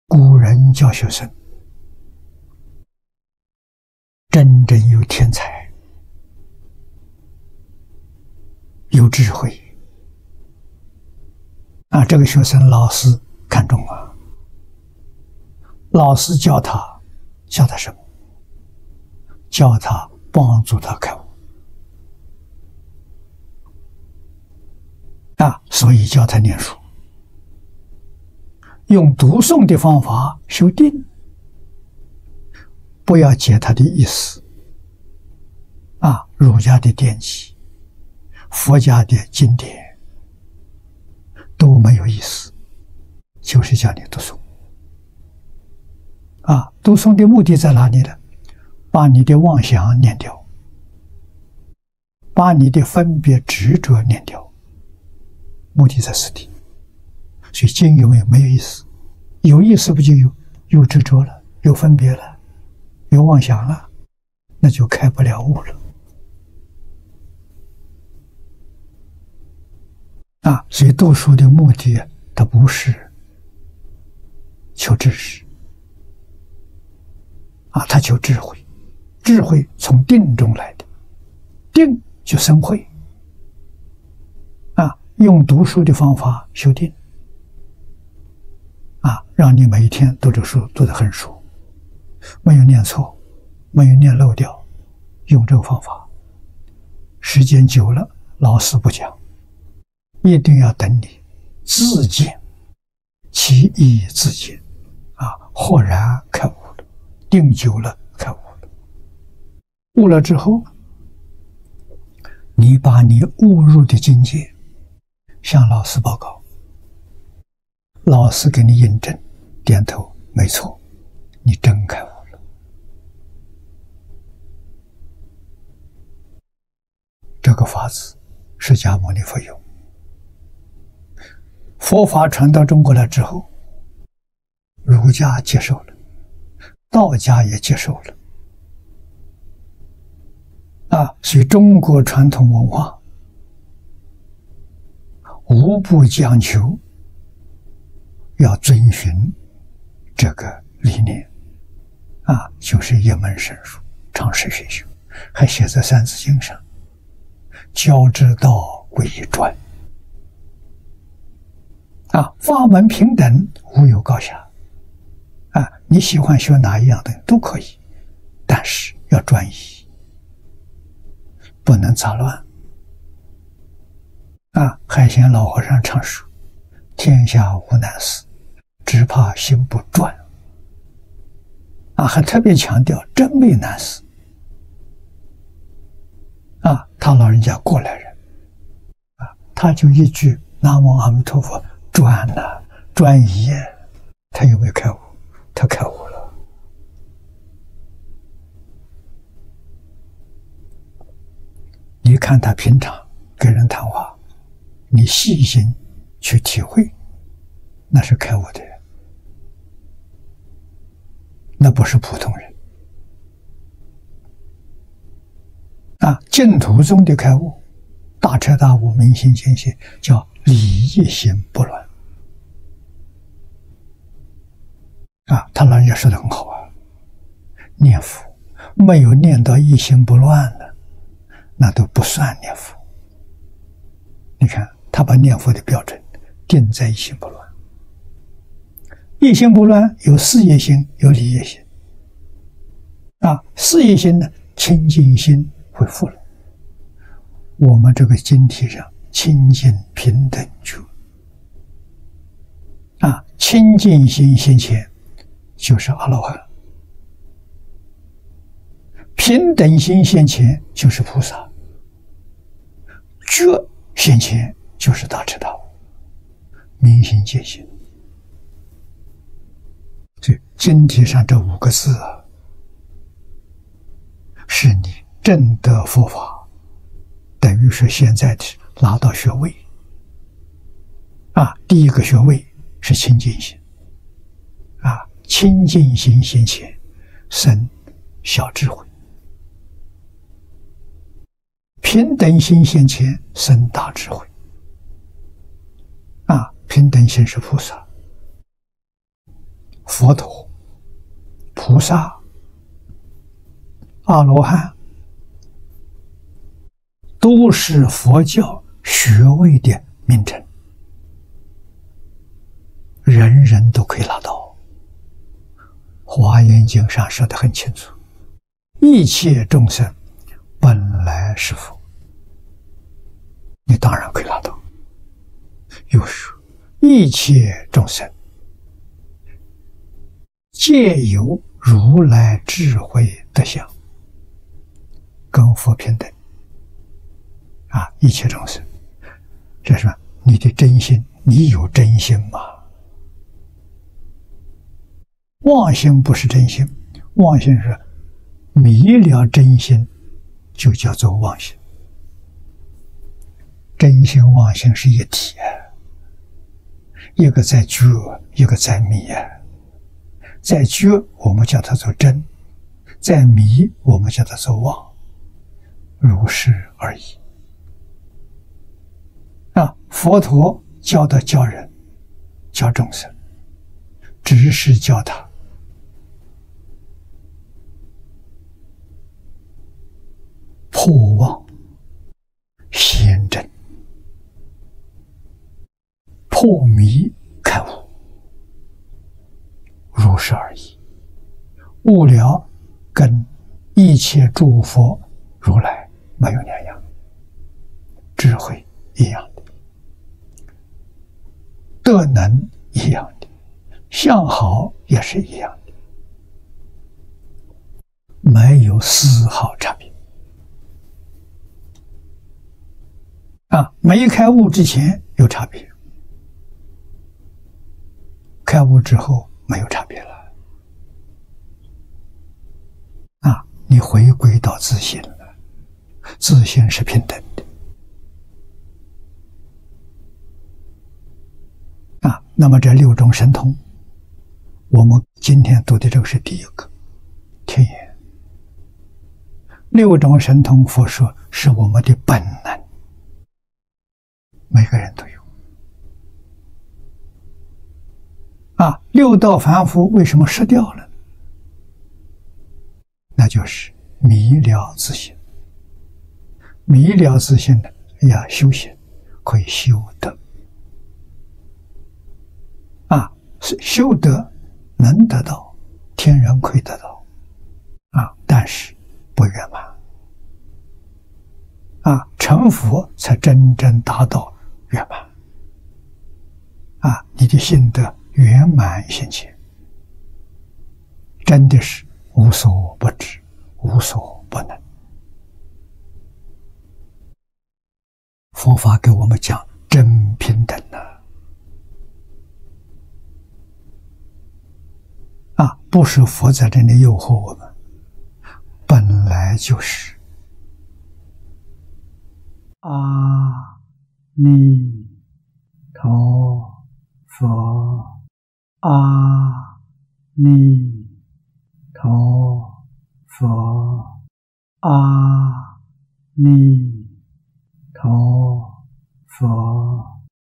🙏淨空老法師🙏這個學生老師看重🙏
古人教學生，
真正有天才、有智慧，這個學生老師看重。老師教他，教他什麼？教他，幫助他開悟。所以教他念書，用讀誦的方法修定，不要解它的意思，儒家的典籍、佛家的經典都沒有意思，就是叫你讀誦。讀誦的目的在哪裡？把你的妄想念掉，把你的分別執著念掉，目的在此地。所以經有沒有意思？沒有意思，有意思不就有執著、有分別、有妄想了嗎？那就開不了悟。所以讀書的目的，它不是求知識，它求智慧。智慧從定中來的，定就生慧。用讀書的方法修定，讓你每天讀這個書讀得很熟，沒有念錯，沒有念漏掉。用這個方法，時間久了，老師不講，一定要等你自見，其義自見，豁然開悟了，定久了開悟，悟了之後你把你悟入的境界向老師報告，老師給你印證，點頭，沒錯，你真開悟了。這個法子，釋迦牟尼佛用。佛法傳到中國來之後，儒家接受了，道家也接受了，所以中國傳統文化無不講求要遵循這個理念，就是「一門深入，長時薰修」，還寫在《三字經》上，「教之道，貴以專」。「法門平等，無有高下」，你喜歡學哪一樣東西都可以，但是要專一，不能雜亂。海賢老和尚常說「天下無難事，只怕心不專」，還特別強調「真沒有難事」。他老人家過來人，他就一句南無阿彌陀佛，專，專一。
　　他有沒有開悟？他開悟了。你看他平常跟人談話，你細心去體會，那是開悟的人，那不是普通人。淨土宗的開悟，大徹大悟，明心見性，叫理一心不亂。他老人家說得很好：「念佛沒有念到一心不亂，那都不算念佛。」你看他把念佛的標準定在一心不亂。一心不亂有事一心，有理一心。事一心，清淨心恢復了。我們這個經題上「清淨平等覺」，清淨心現前就是阿羅漢，平等心現前就是菩薩，覺現前就是大徹大悟，明心見性。所以經題上這五個字是你證得佛法，等於說現在的是拿到學位，第一個學位是清淨心，清淨心現前生小智慧，平等心現前生大智慧，平等心是菩薩。佛陀、菩薩、阿羅漢都是佛教學位的名稱，人人都可以拿到。《華嚴經》上說得很清楚，「一切眾生本來是佛」，你當然可以拿到；又說，「一切眾生皆有如來智慧德相」，跟佛平等，一切眾生。這是什麼？你的真心，你有真心！妄心不是真心，妄心是迷了真心就叫做妄心。真心、妄心是一體，一個在覺，一個在迷，在覺我們叫它做真，在迷我們叫它做妄，如是而已。佛陀教人、教眾生，只是教他破妄顯真，破迷開悟，如是而已。悟了，跟一切諸佛如來沒有兩樣，智慧一樣的，德能一樣的，相好也是一樣的，沒有絲毫差別。沒開悟之前有差別，開悟之後沒有差別，你回歸到自性，自性是平等的。這六種神通，我們今天讀的這是第一個，天眼。六種神通，佛說是我們的本能，每個人都有。六道凡夫為什麼失掉？那就是迷了自性。迷了自性，你要修行，可以修得。修得，能得到，天人可以得到，但是不圓滿，成佛才真正達到圓滿。你的性德圓滿現前，真的是無所不知、無所不能。佛法給我們講真平等，不是佛在這裡誘惑我們，本來就是。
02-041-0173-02（這個學生老師看重）《淨土大經科註（第四回）》(第173集)00:18:49-00:28:01
至誠感恩 ! 🙏❤🙏❤🙏❤